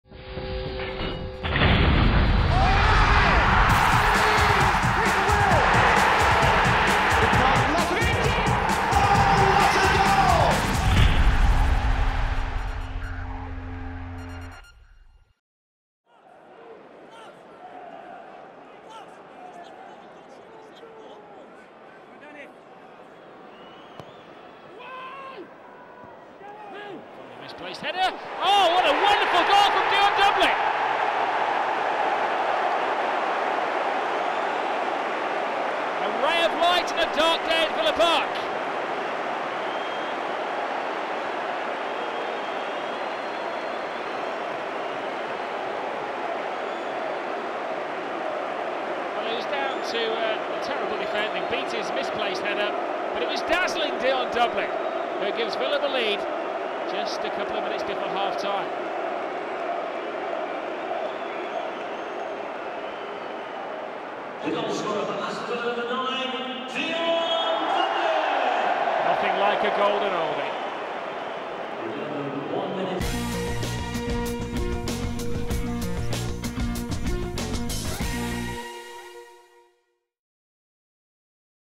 Dion Dublin's over-head kick against Southampton
Ahead of tonight's clash with Southampton, relive Dion Dublin's fantastic goal against The Saints at Villa Park in November 2003.